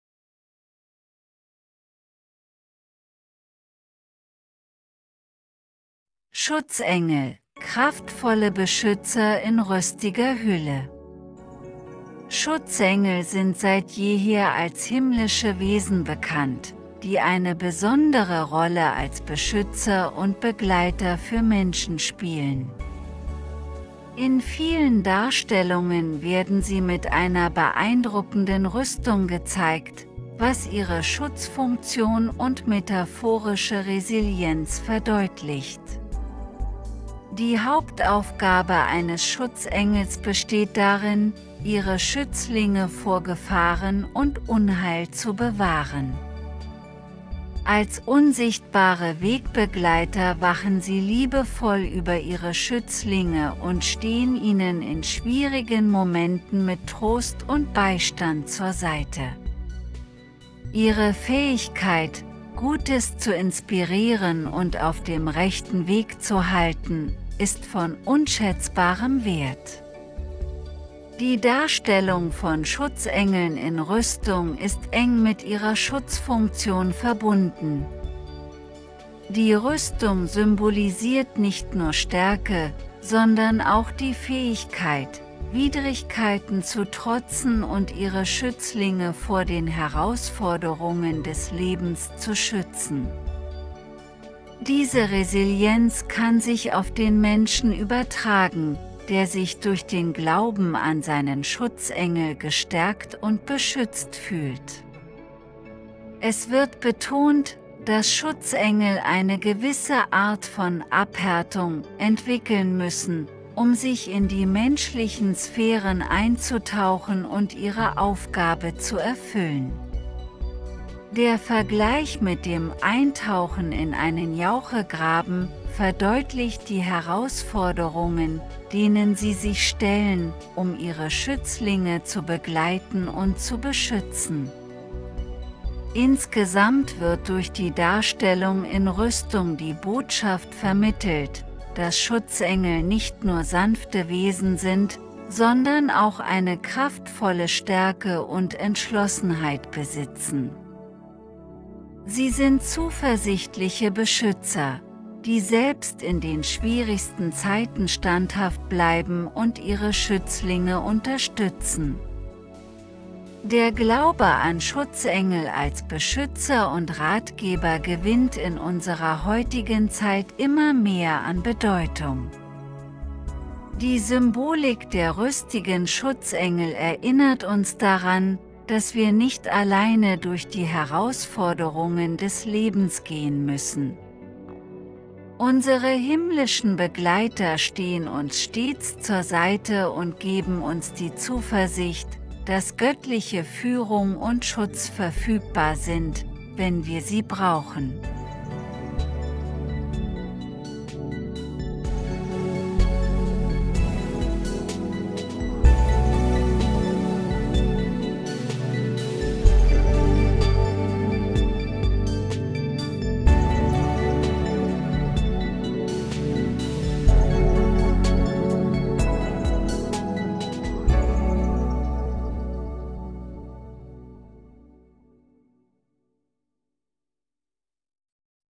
Schutzengel - Kraftvolle Beschützer in rüstiger Hülle
Willkommen zu unserem faszinierenden Youtube-Video "Schutzengel - Kraftvolle Beschützer in rüstiger Hülle"! Tauche ein in die fesselnde Welt der himmlischen Wesen, die seit jeher als treue Begleiter und Beschützer der Menschen gelten.

Diese Bilder stammen aus der "Werkstatt" von meinem Sohn Ricardo und wurde von mir in meinem persönlichen Stil ausgemalt.

In diesem Video erforschen wir die faszinierende Rolle von Schutzengeln und die Symbolik ihrer beeindruckenden Rüstung. Erfahre mehr über ihre bedeutsamen Aufgaben und ihre kraftvolle Präsenz, die viele Menschen auf der ganzen Welt inspiriert und ihnen Trost spendet.

Unsere Reise beginnt mit einem Blick auf die Hauptaufgabe der Schutzengel: den Schutz ihrer Schützlinge vor Gefahren und Unheil. Wir enthüllen, wie sie als unsichtbare Hüter stets über uns wachen und in schwierigen Momenten liebevoll Beistand leisten. Ihre Fähigkeit, Gutes zu inspirieren und auf dem rechten Weg zu halten, ist von unschätzbarem Wert für jeden von uns.

Die visuelle Darstellung von Schutzengeln in Rüstung ist faszinierend und fesselnd zugleich. Wir zeigen dir, wie diese Rüstung ihre Schutzfunktion betont und eine metaphorische Resilienz symbolisiert. Entdecke, wie diese beeindruckende Symbolik sich auf uns als Menschen übertragen kann und uns in schwierigen Zeiten stärkt und beschützt.

Tauche weiter ein, wenn wir die Notwendigkeit einer gewissen "Abhärtung" für Schutzengel erforschen. Ihre Mission, sich in die menschlichen Sphären einzutauchen, wird oft mit dem Eintauchen in einen Jauchegraben verglichen, um ihre Schützlinge zu begleiten und zu beschützen. Wir erklären, wie diese Herausforderungen ihre Entschlossenheit und Standhaftigkeit als Beschützer betonen.

Unser Video vermittelt eine inspirierende Botschaft über den Glauben an Schutzengel als wertvolle Begleiter und Ratgeber. Ihre Rüstung symbolisiert nicht nur Stärke, sondern auch die Zuversicht, dass wir niemals alleine durch die Herausforderungen des Lebens gehen müssen. Unsere himmlischen Begleiter stehen uns immer zur Seite und bieten göttliche Führung und Schutz, wenn wir sie brauchen.

Begleite uns auf dieser spannenden Reise in die Welt der Schutzengel und lasse dich von ihrer kraftvollen Präsenz und ihrer bedeutsamen Rolle als Beschützer inspirieren. Klicke jetzt auf das Video und tauche ein in die zauberhafte Welt der himmlischen Hüter - "Schutzengel - Kraftvolle Beschützer in rüstiger Hülle"! Viel Spaß!